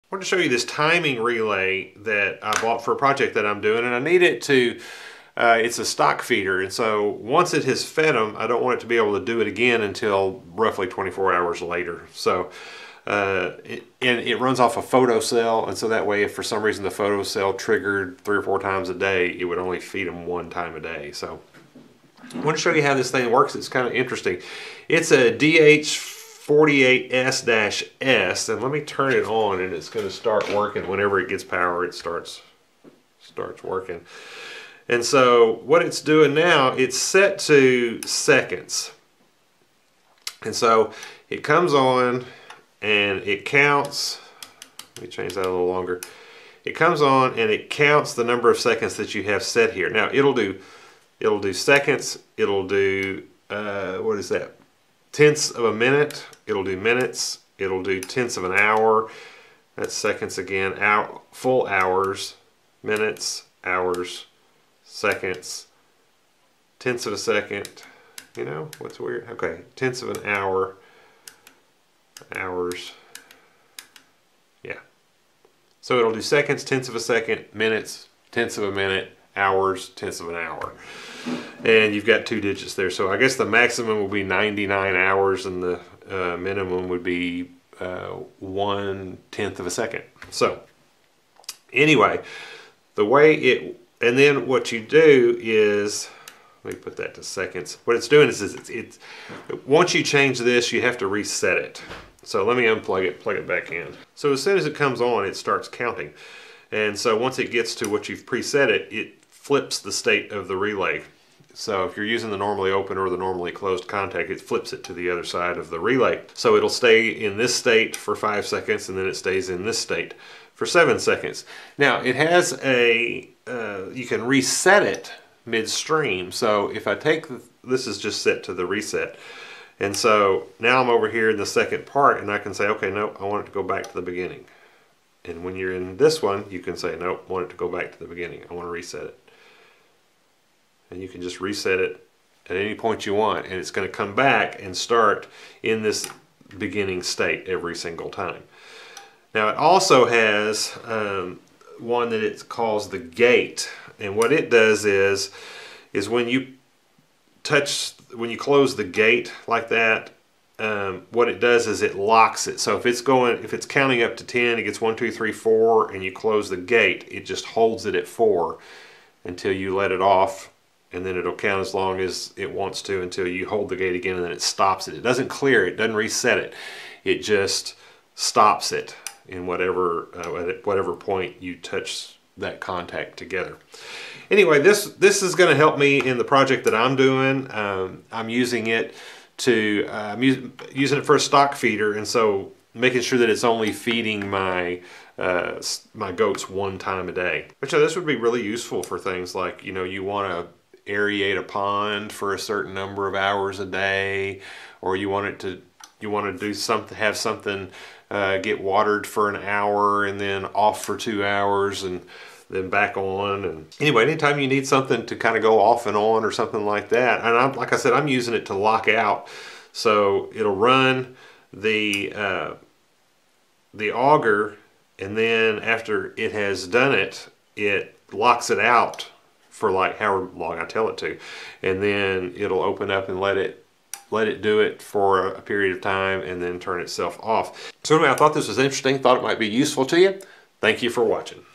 DH48S Timing Relay
How the DH48S timing relay works.

Timing relay Probably cheaper on ebay search for DH48S-S